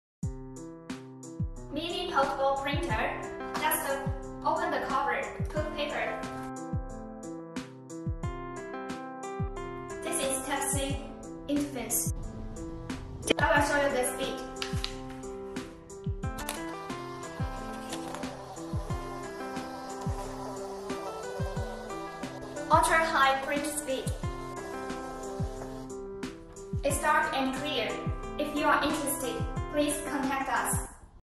mini portable thermal printer--beautiful design -HOP-Q200 |HOIN
This product support multiple language print,it's so portable! You can use it everywhere!

HOP-HQ200 is adesigned mobile receipt printer with high-performance.

58mm portable thermal printer

●2000MAH Li-on battery,Charger: 5V/1A

●70 mm / s ultra high print speed

●Support 1D+2D  bar code printing

●Tyoe C USB charging and data transmission

●support 40MM large diameter rolls

●support GB18030 big font

●support multi language print